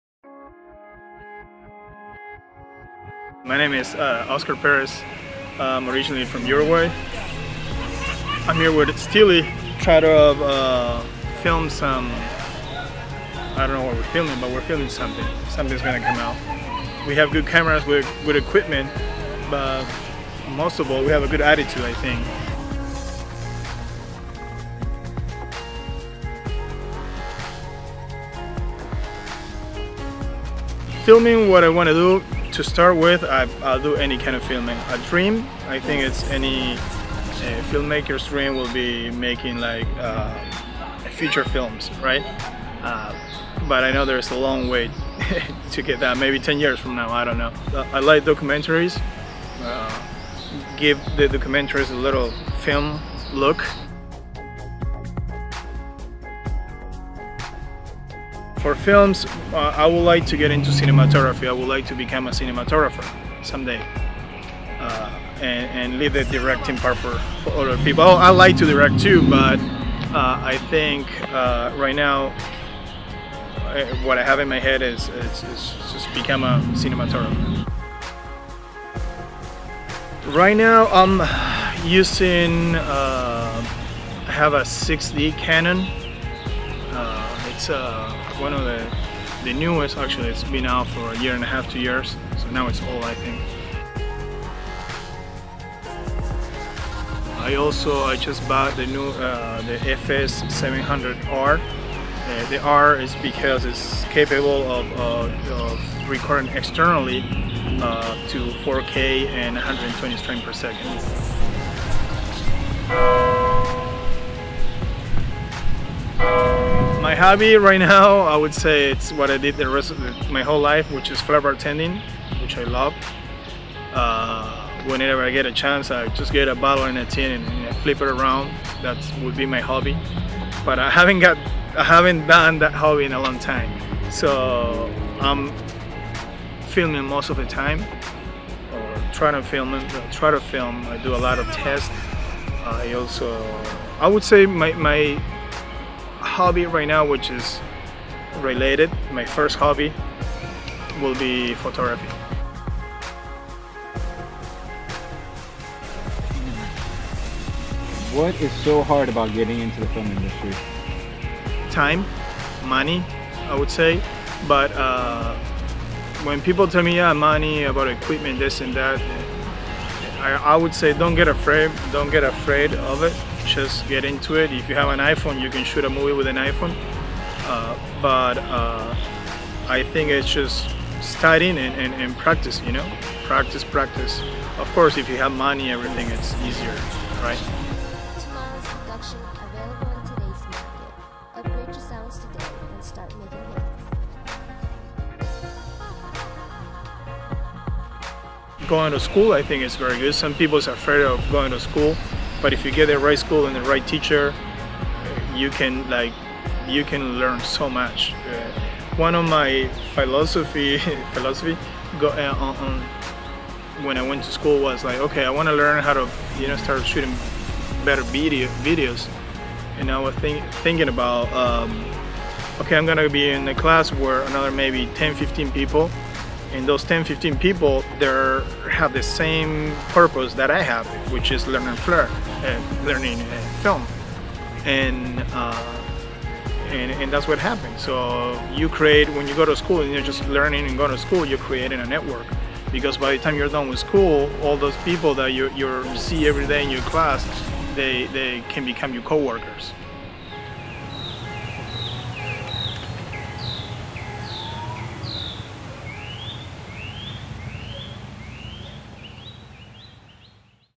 Osar Perez - Cinematographer in Training.
A quick bio video about Oscar Perez.